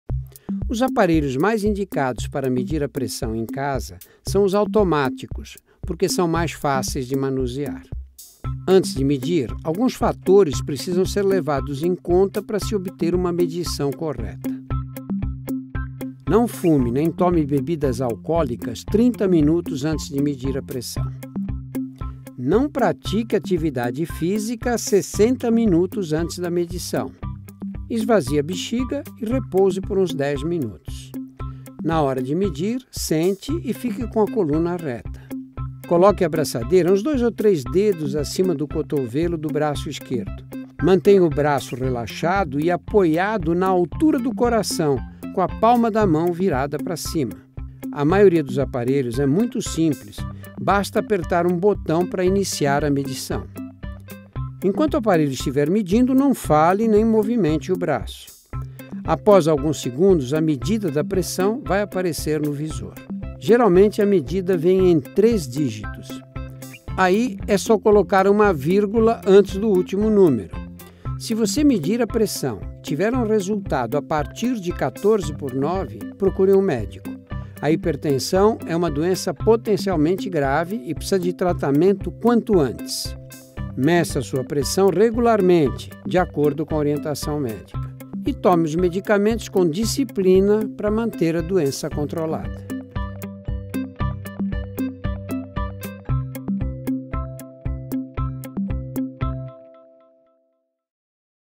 Drauzio Varella - Dicas Práticas - Como medir sua pressão
Você sabe como medir a sua pressão? Existem algumas regrinhas bem importantes que você deve seguir para ter o resultado exato. Para começar: você sabia que o repouso de pelo menos 10 minutos é essencial para a medição? Então vem ver outras dicas fundamentais para você.